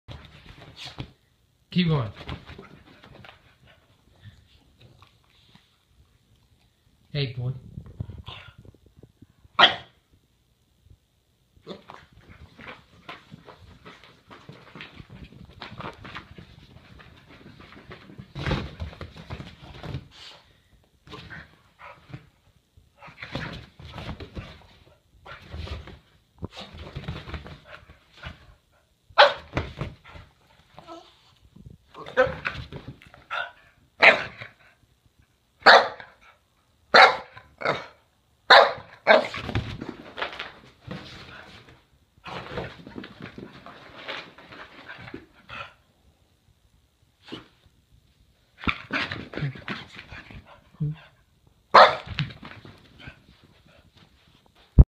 Boston Terrier doing zoomies wanting to play!
Boston terrier (Toro) wanting to play